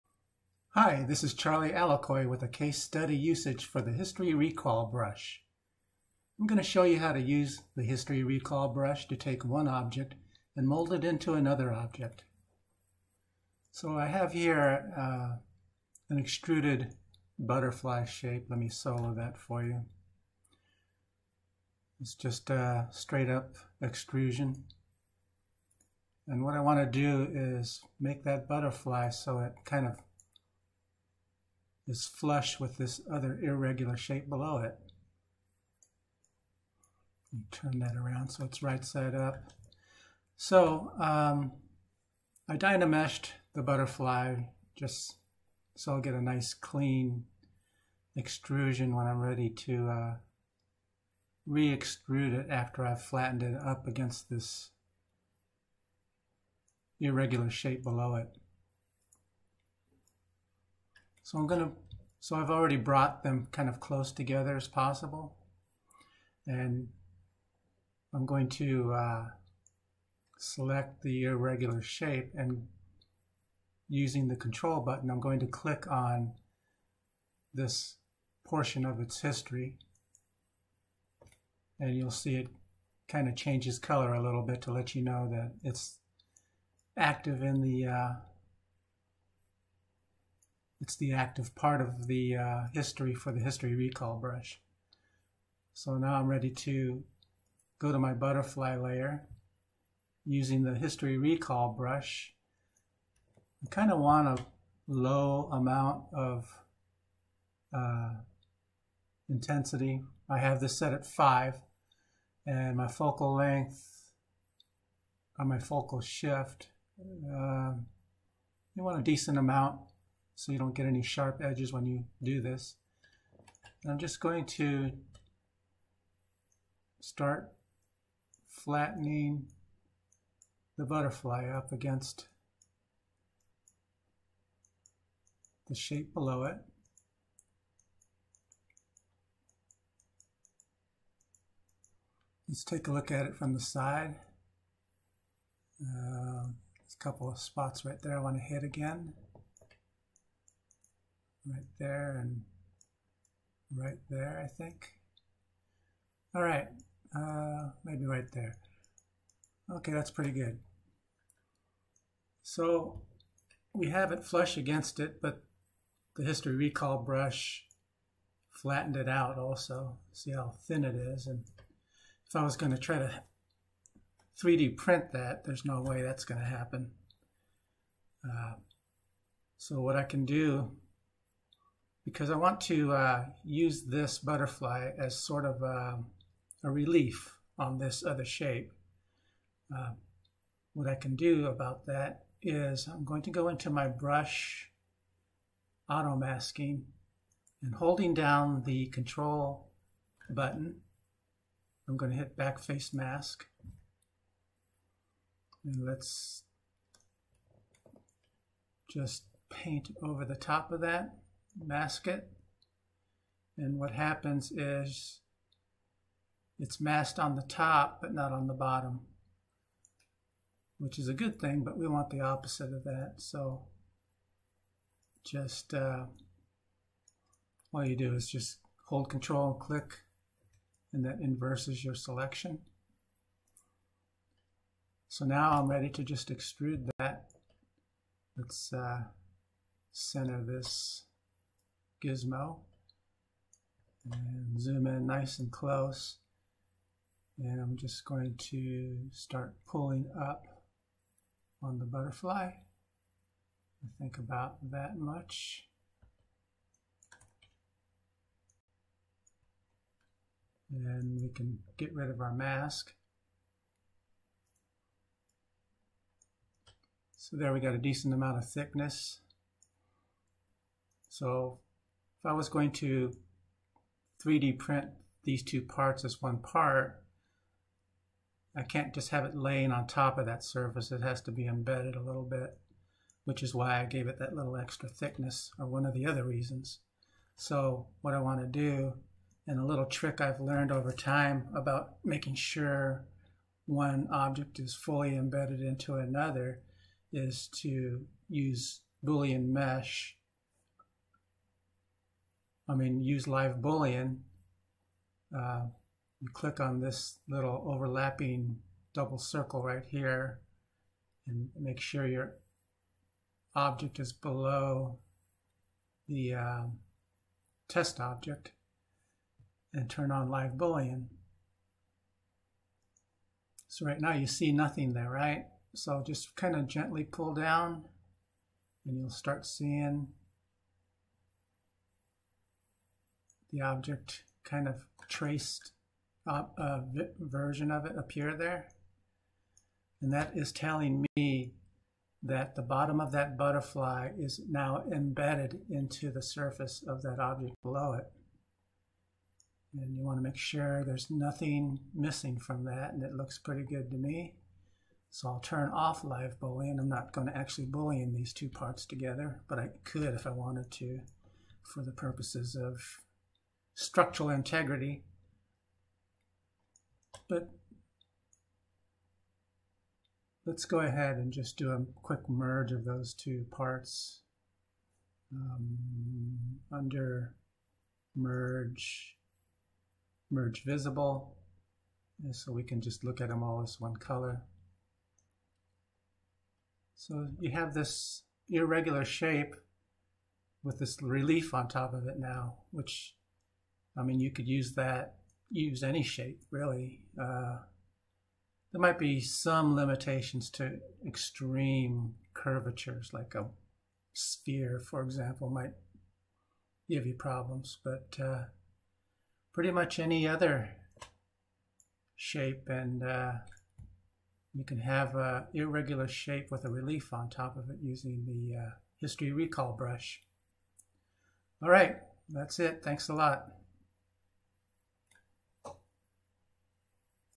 Zbrush 2020 History Recall Brush Case Study
How to use the history recall brush to add relief onto an irregular or curved surface.